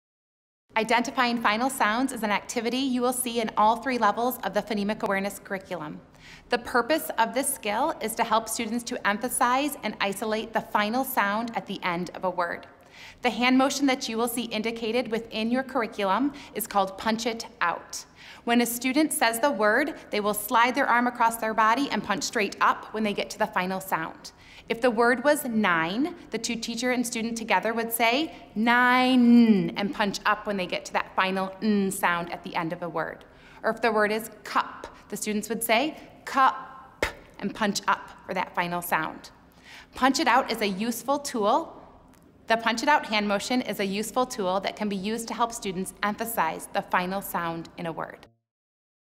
Final Sounds Hand Motion for Heggerty Phonemic Awareness
This video demonstrates the final sounds hand motion that is listed in the Heggerty Phonemic Awareness Curriculum. Alisa models blending words, syllables, onset-rime, and sounds.